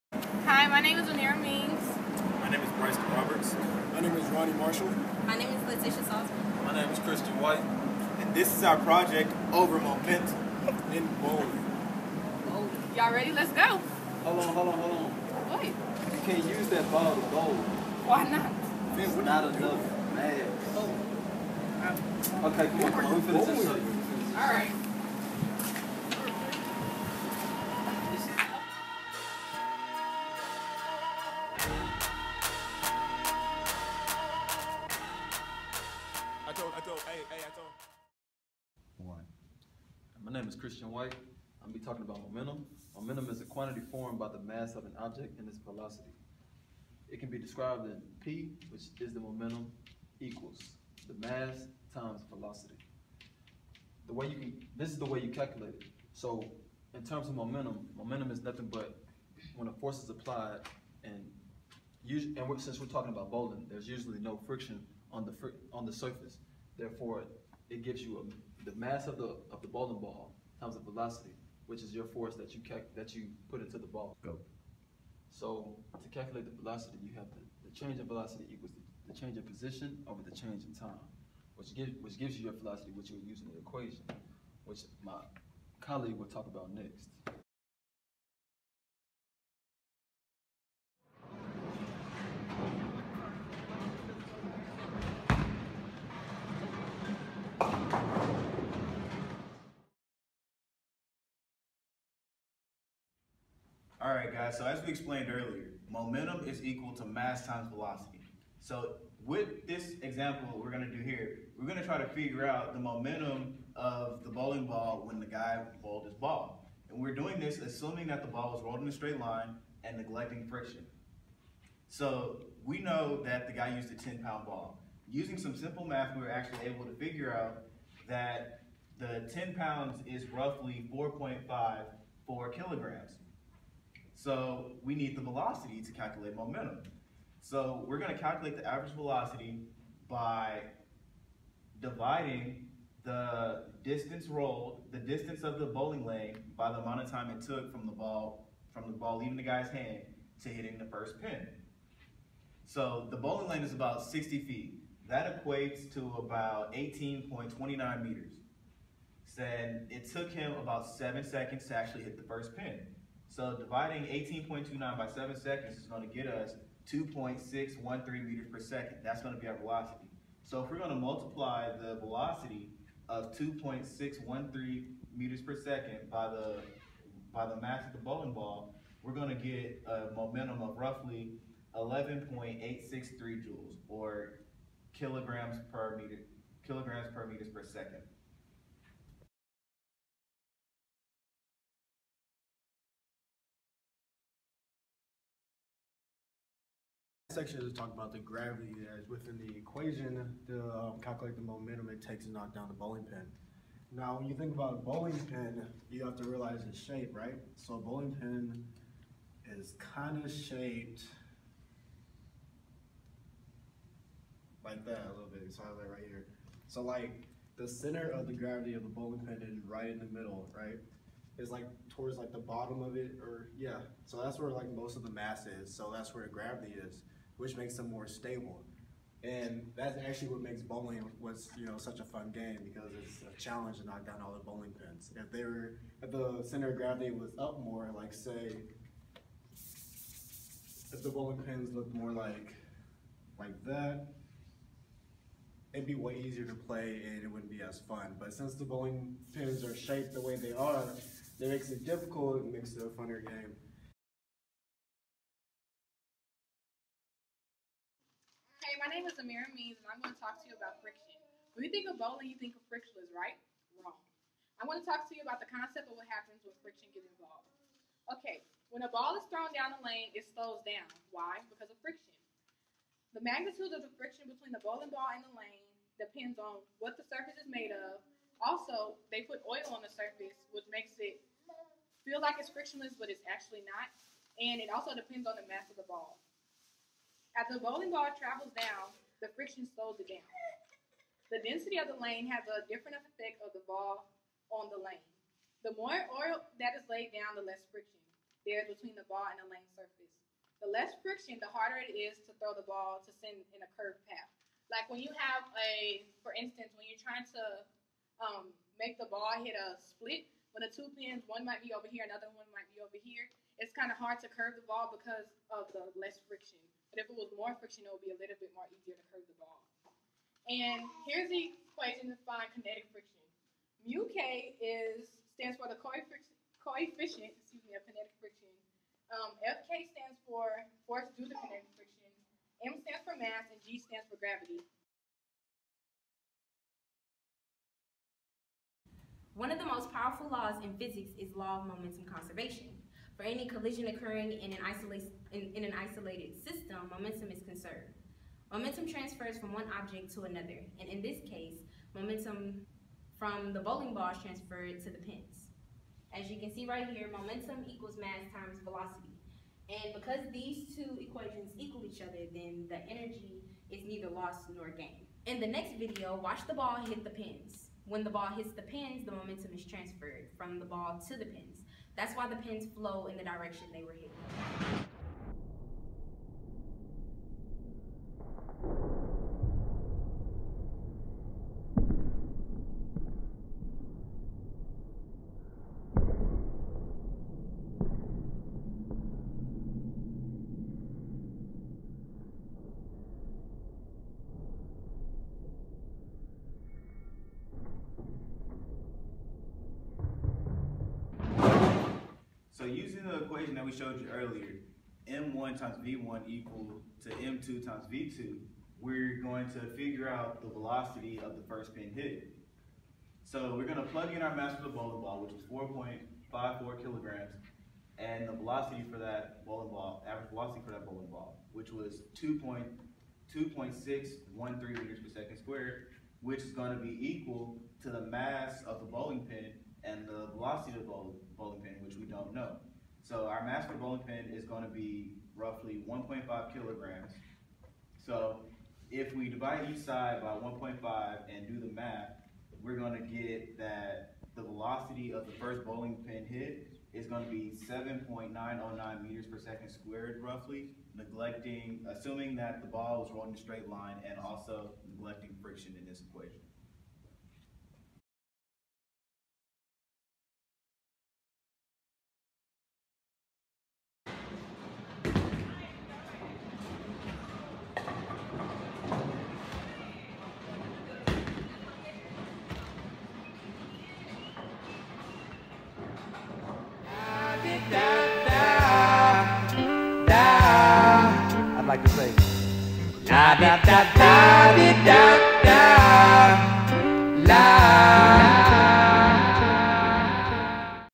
Momentum in Bowling (Physics Project)
Physics Project on Momentum by: Bryston Roberts, LaTisha Salsman, Amirah Means, Christian White, Reggie Marshall